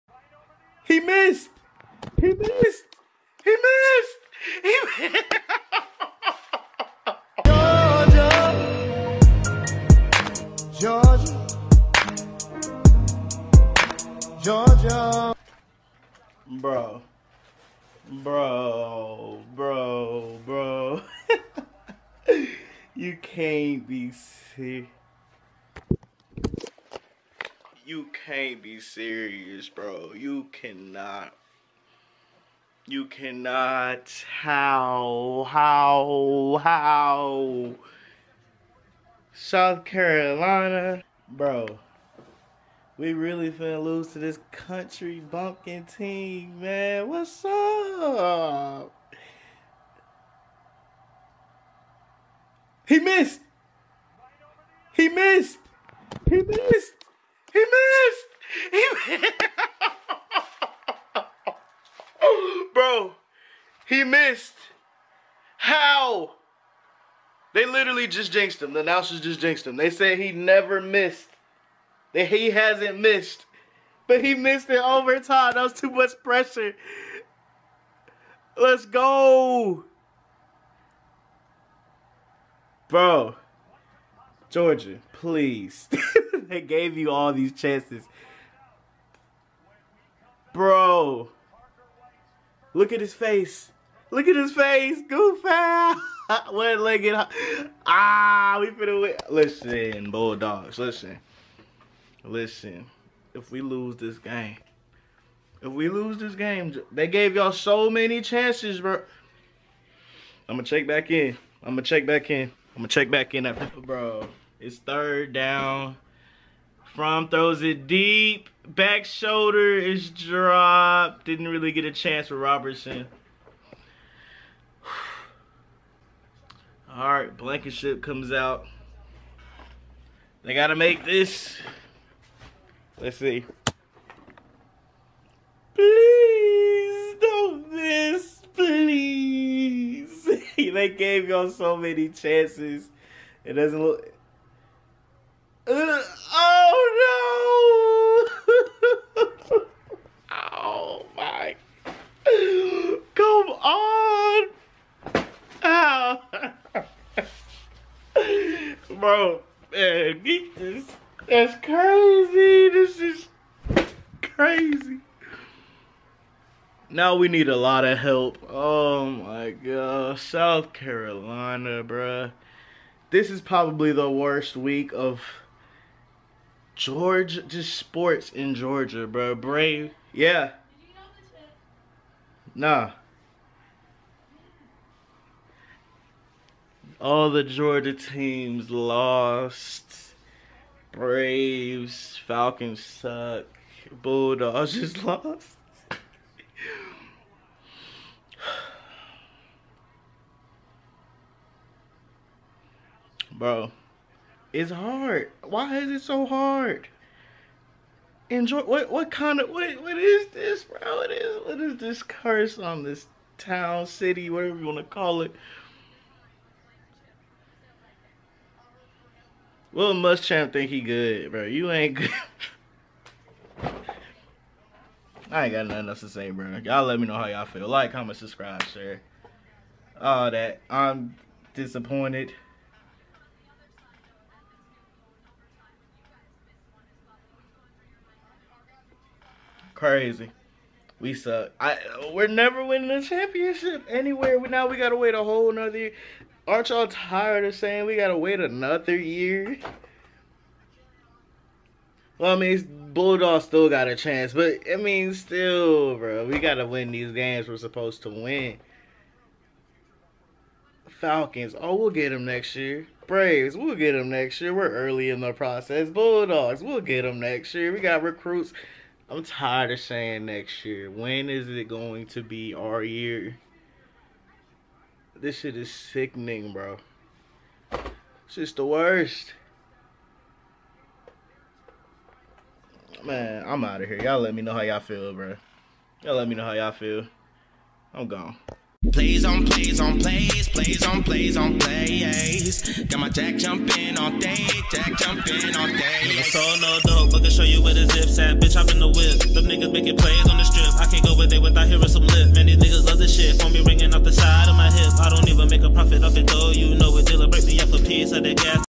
ANGRY GEORGIA FANS RAGE ABOUT LOSS SOUTH CAROLINA 20 - GEORGIA 17
ANOTHER DISAPPOINTING LOSS IN THE STATE OF GEORGIA